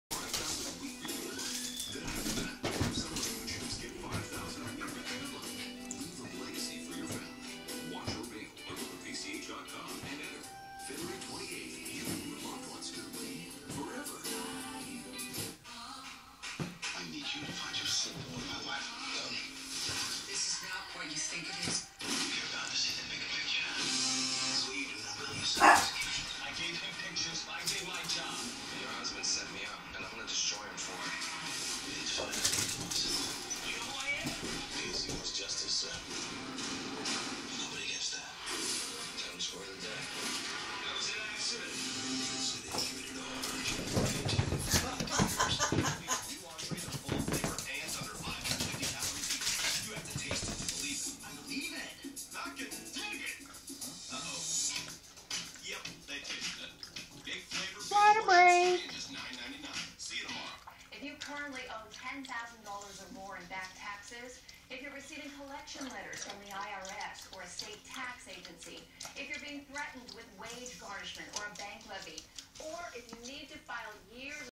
Cat chase chihuahua!
Pillin chases choco. This is a prime example of cats & dog getting along.